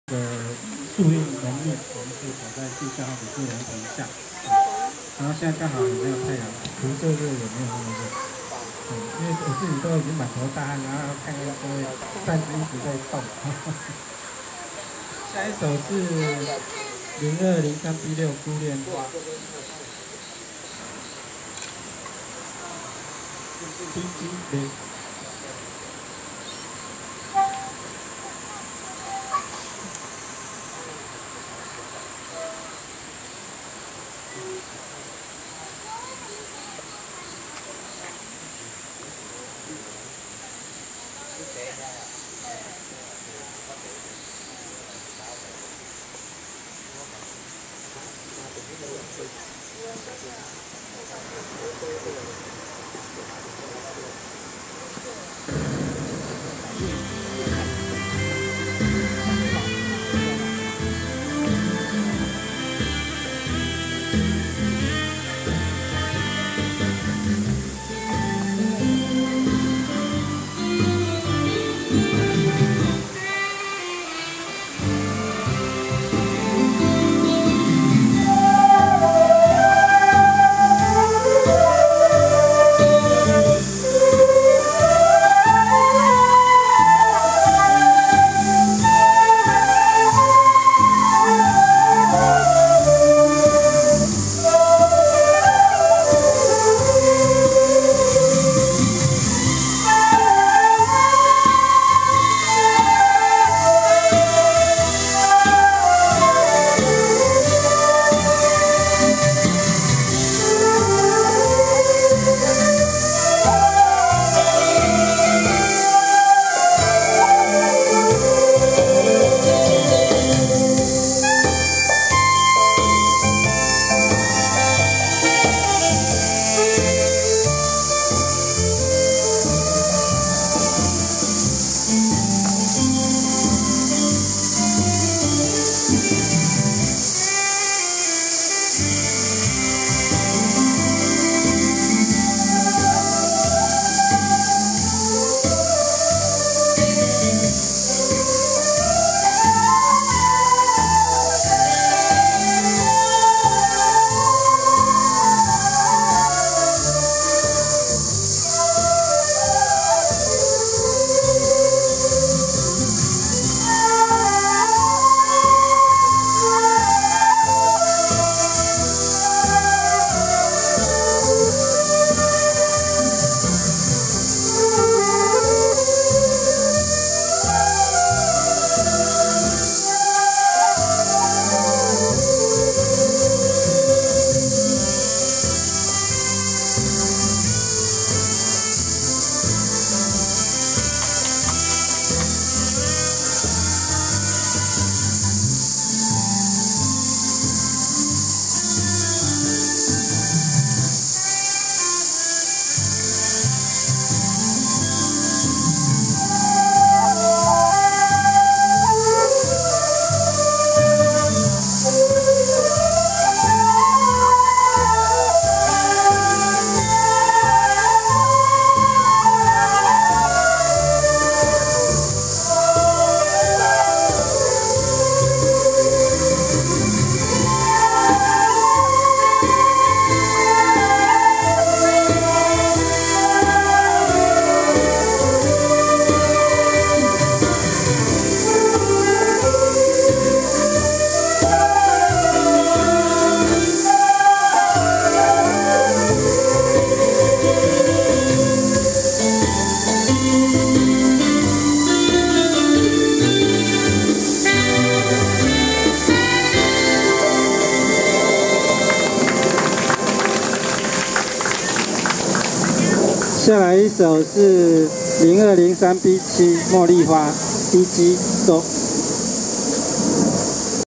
信義社大蘇文宗師生笛簫虎山溪畔音樂會MOV06e.MOD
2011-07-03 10:00 ~ 16:00溪谷中的簫笛聲 蘇文宗老師與信義社大［竹韻簫聲］［陶笛之聲］戶外音樂會！！